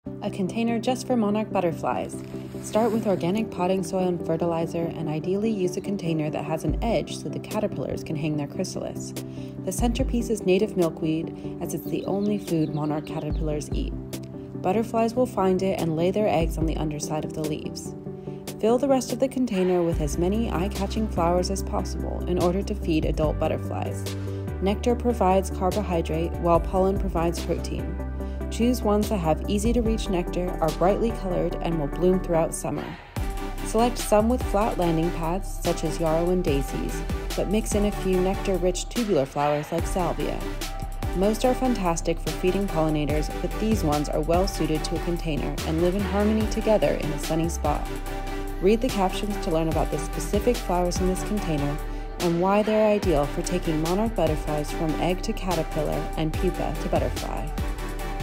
An EASY Patio Container Garden for Monarch Butterflies (and caterpillars)
Raise a monarch butterfly with this simple butterfly habitat you can grow in a container 👇

🚜🦠Foundations: good soil is a huge part of the ecosystem. Mycorrhizae & beneficial bacteria are essential to creating a living garden. Your butterfly garden starts from the bottom up with organic soil, worm castings, and starter fertilizer.

🥛🌱Milkweed: insects have evolved alongside plants for millennia. Monarch caterpillars exclusively eat milkweed. Why? This plant is actually toxic to many birds and bugs - caterpillars are a scrumptious protein rich treat for hungry birds so they have evolved to eat a food source that makes them toxic to their predators! Caterpillars live up to their reputation and are hungry, hungry indeed. 🐛Provide as much milkweed as you can get your hands on. Choose a variety native to your region so you don’t have to worry about when to cut it back.

🌸Flowers: provide a variety of flower colors, shapes & sizes to appeal to an assortment of beneficial bugs acd provide necessary nectar for adult Monarch Butterflies.

Ask all your burning butterfly & garden questions in the comments or just leave a 🦋 if you enjoyed this video!